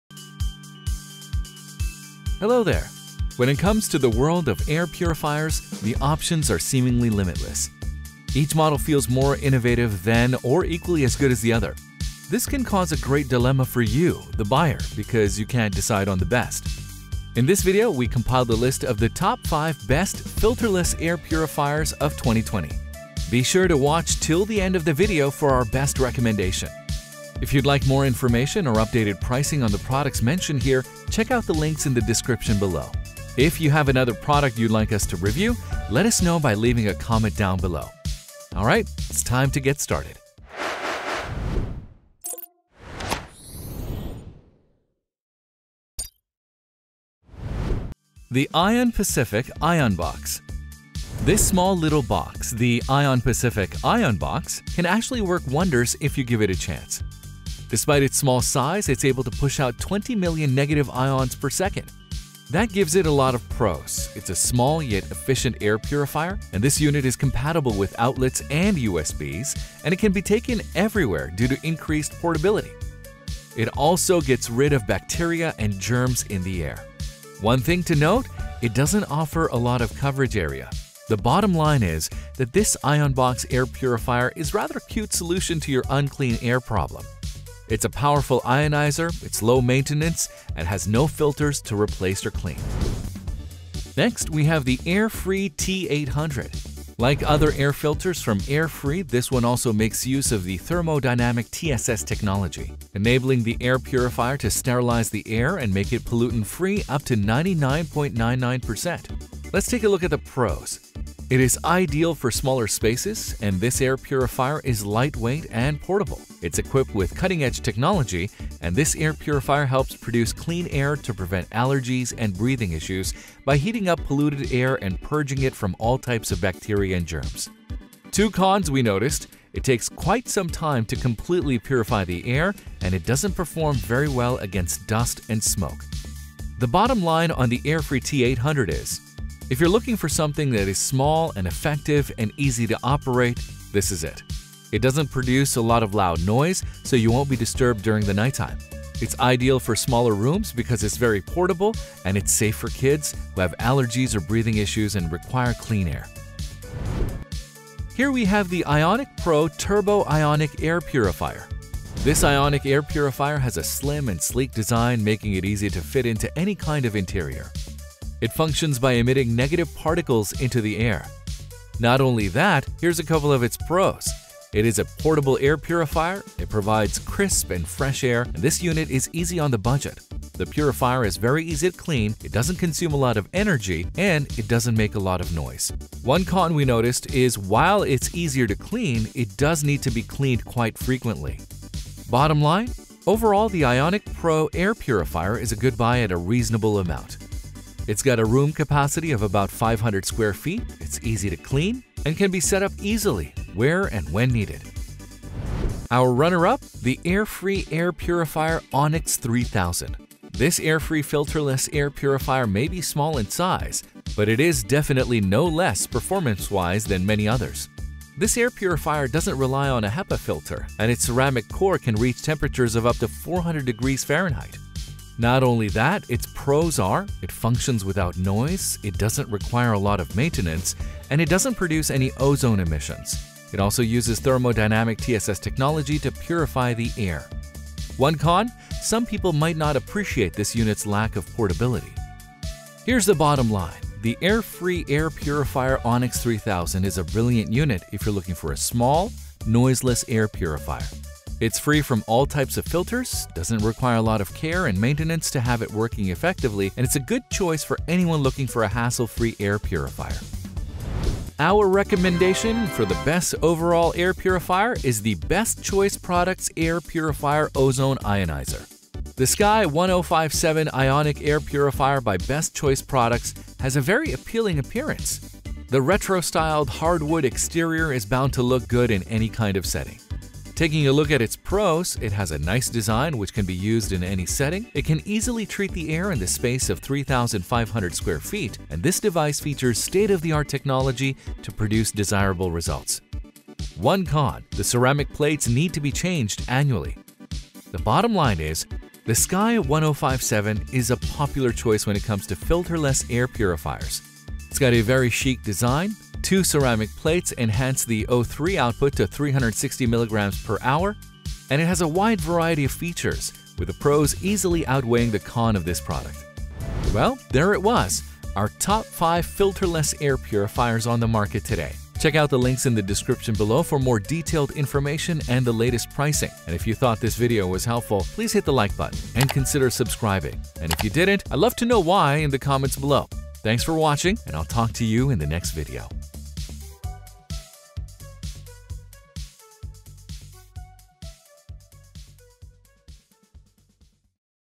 Best Filterless Air Purifiers ⬜: Your Guide to the Best Options | HVAC Training 101
(Best Filterless Air Purifiers)

✅ Featured Products for Best Filterless Air Purifiers

0:00 Introduction
0:49 5.IonPacific Ionbox ➜
1:35 4.Airfree T800 ➜
2:39 3.Ionic Pro 90IP01TA01W Turbo Ionic Air Purifier ➜
3:27 2.Airfree Air Purifier Onix3000 ➜
4:25 1.Best Choice Products Air Purifier Ozone Ionizer ➜

⬜ Filterless air purifiers, as the name suggests, don’t make use of filters at all. Instead of using filters, these air purifiers emit a charge which latches onto the contaminants in the air.

⬜ After the particles are charged, they are then gathered on an oppositely charged plate. This works very differently than filter-based air purifiers which usually filter the polluted air and output the clean air.

💬 Let us know what you think of these Filterless Air Purifiers in the comment section below.